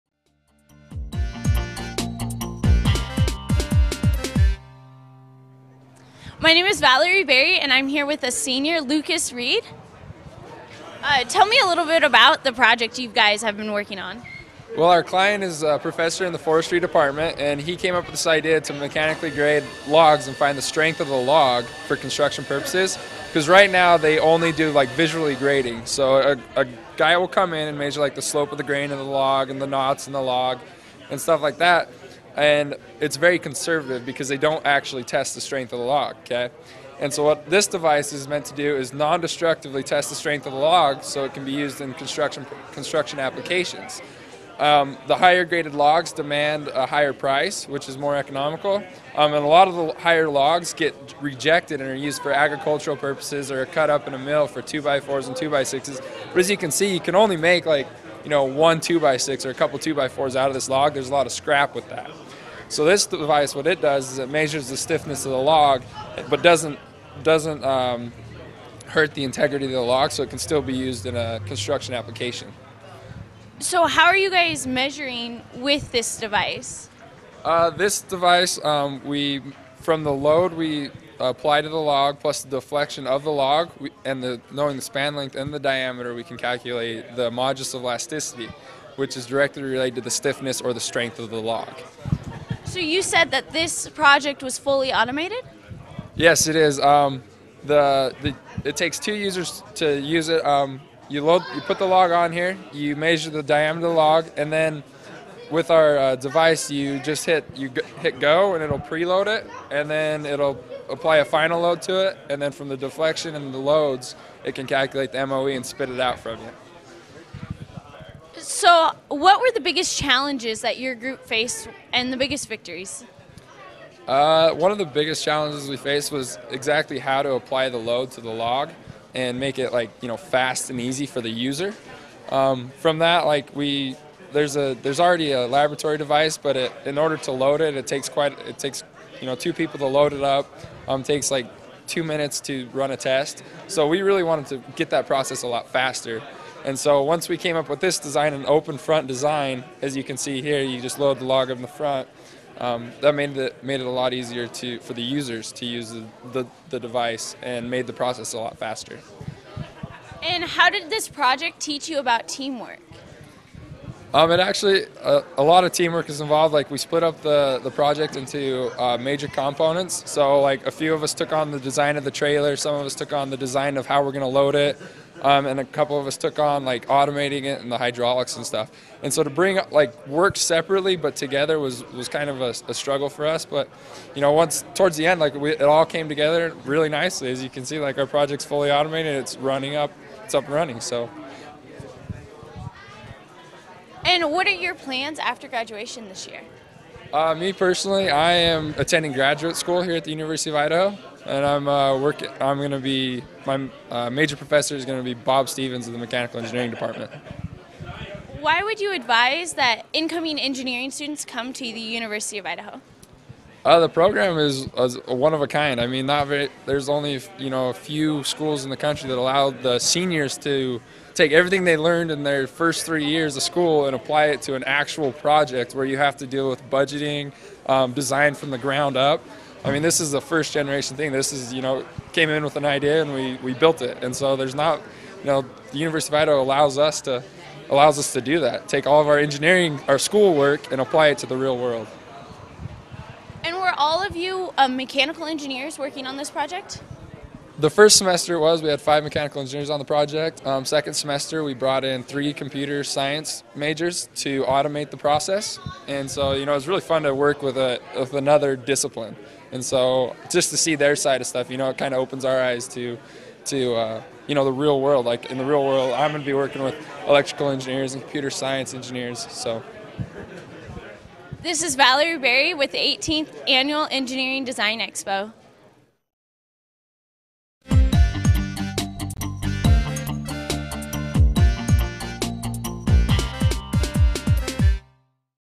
Portable Timber Grading Mechanism - 2011 EXPO Interview
Team Flexgrade designed and fabricated a mobile apparatus to mechanically grade round timber that is to be used as integral parts of structures.

Currently, round timber is visually graded. The team has developed a machine to do this more efficiently.